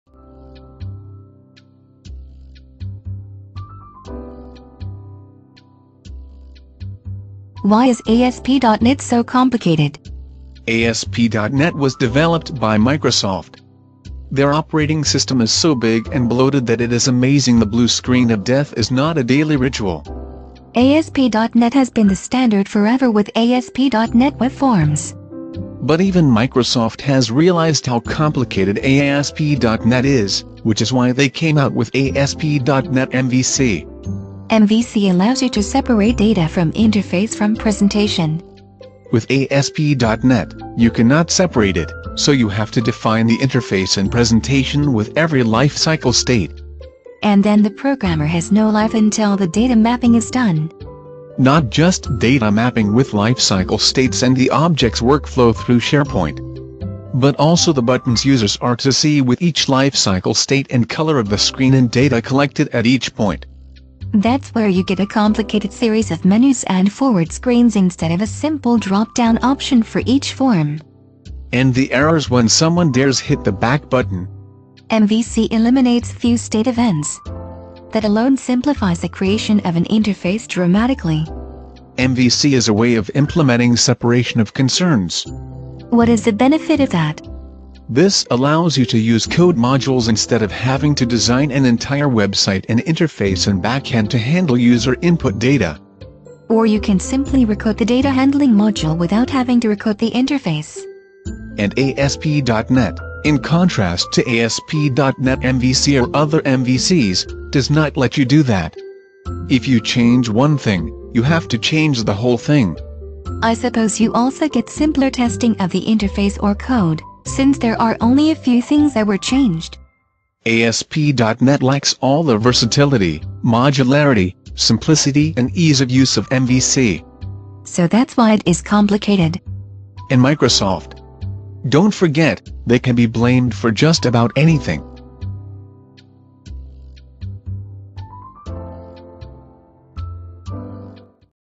Why is ASP.NET So Complicated?
ASP.net was developed by Microsoft. Their operating system is so big and bloated that it is amazing the blue screen of death is not a daily ritual.

ASP.net has been the standard forever with ASP.net web forms.

But even Microsoft has realized how complicated ASP.net is, which is why they came out with ASP.net MVC.

MVC allows you to separate data from interface from presentation.

With ASP.net, you can not separate it, so you have to define the interface and presentation with every lifecycle state.

And then the programmer has no life until the data mapping is done.

Not just data mapping with life cycle states and the object's workflow through Sharepoint, but also the buttons users are to see with each life cycle state and color of the screen and data collected at each point.

That's where you get a complicated series of menus and forward screens instead of a simple drop down option for each form.

And the errors when someone dares hit the back button.

MVC eliminates view-state events. That alone simplifies the creation of an interface dramatically.

MVC is a way of implementing separation of concerns.

What is the benefit of that?

This allows you to use code modules instead of having to design an entire website and interface and backend to handle user input data.

Or you can simply recode the data handling module without having to recode the interface.

And ASP.net, in contrast to ASP.net MVC or other MVCs, does not let you do that. If you change one thing, you have to change the whole thing.

I suppose you also get simpler testing of the interface or code, since there are only a few things that were changed.

ASP.net lacks all the versatility, modularity, simplicity and ease of use of MVC.

So that's why it is complicated.

And Microsoft. Don't forget, they can be blamed for just about anything.